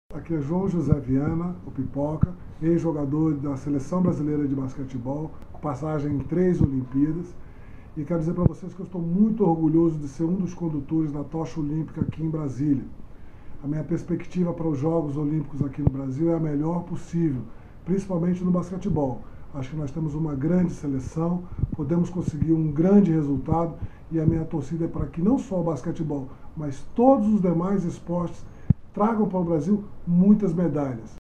Ex-jogador de basquete Pipoka fala sobre o orgulho de carregar a Tocha Olímpica
ESPECIAL WEB - 05.05.16: O ex-jogador da seleção brasileira de basquete João José Vianna, conhecido como Pipoka, fala sobre orgulho de ser escolhido para carregar a Tocha Olímpica em seu trajeto no Brasil.